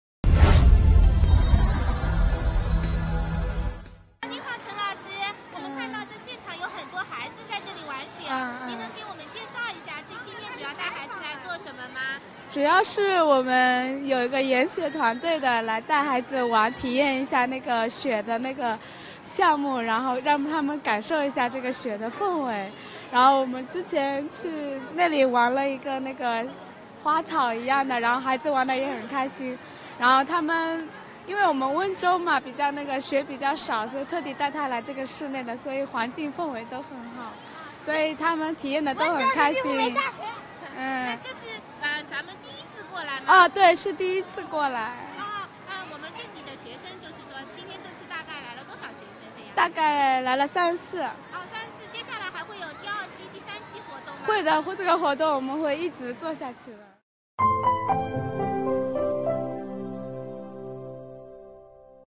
欢乐不停！南方学生室内体验冰雪氛围
在中国新闻网与全球百家华文媒体联合推出的“冬奥24小时”全球全媒体大型直播中，一名教师带领学生们来到室内冰雪场，体验冰雪氛围。